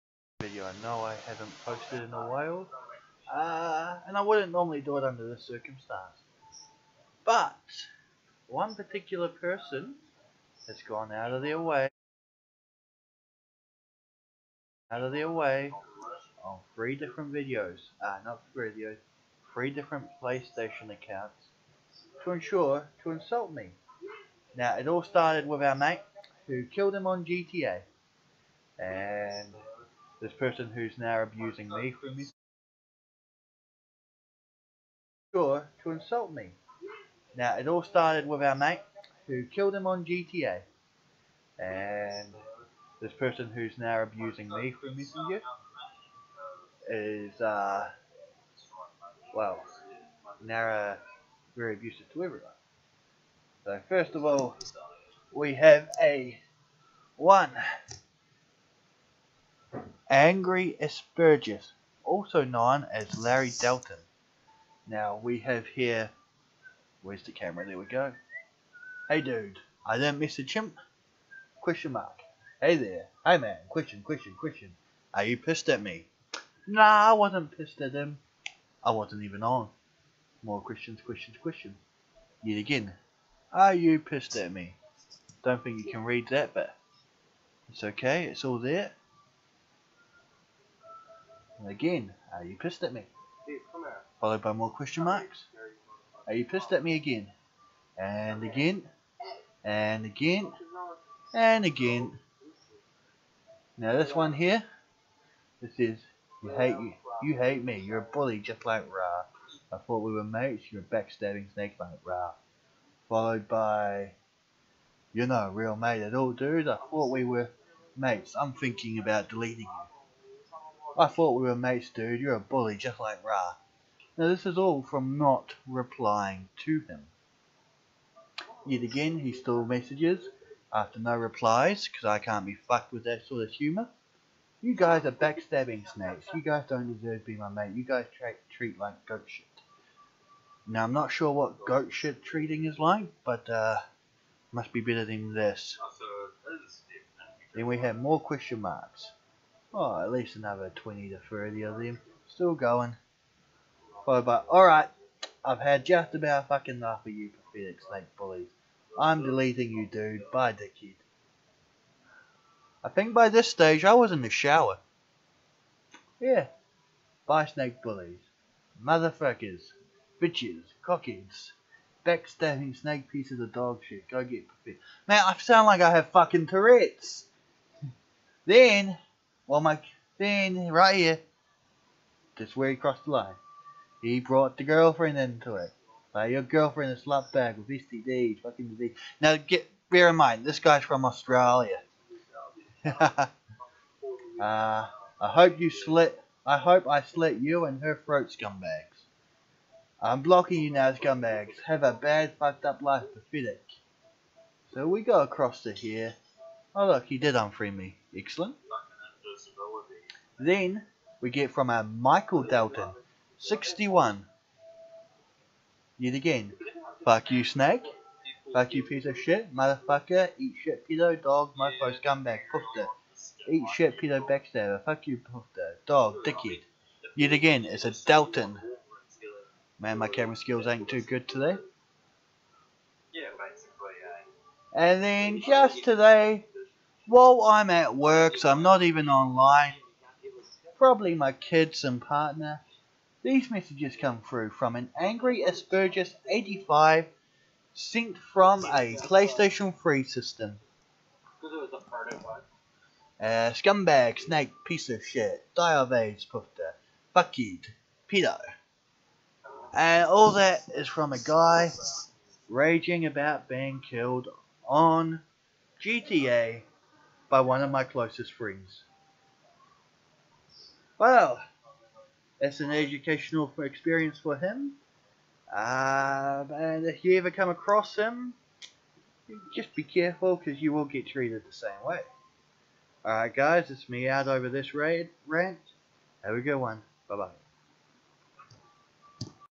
Hate from angry aspergis and his other accounts Warning Abusive Language used
Sorry for the bad editing haha still getting use to it.
This Video is tough language just a warning to others about this current Accounts seen in the Video he got made at me for not saying Hello back to him whilst i was not activve myself  on  the ps4 other accounts he  used are AngryAspergers85 (PS3 Account) PS4 Accounts are MichaelDalton61 and angryaspergis also known as Larry Dalton we were friends on here until now